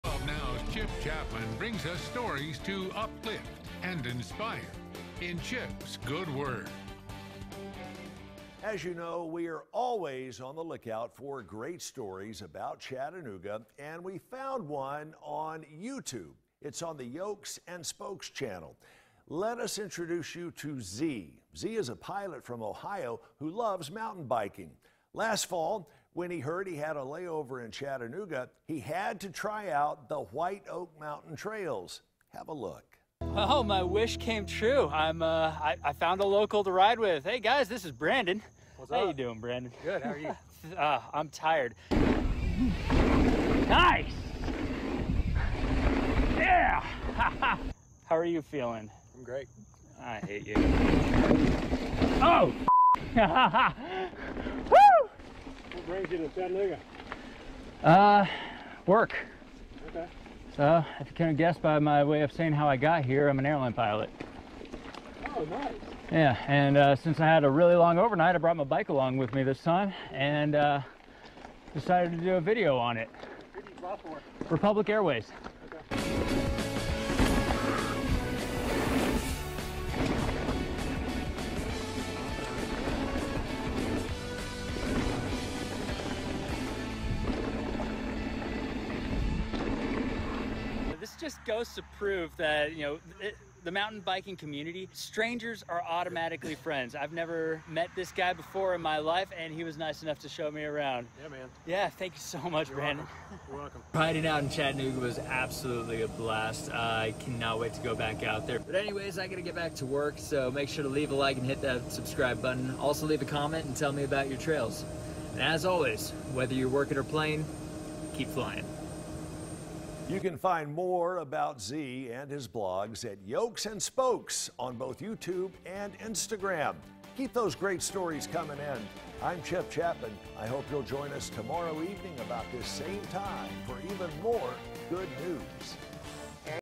Chip's Good Word on Mountain Biking
Chip Chapman shares the story of an airline pilot with a layover in Chattanooga, who brought his mountain bike along to ride some of our trails.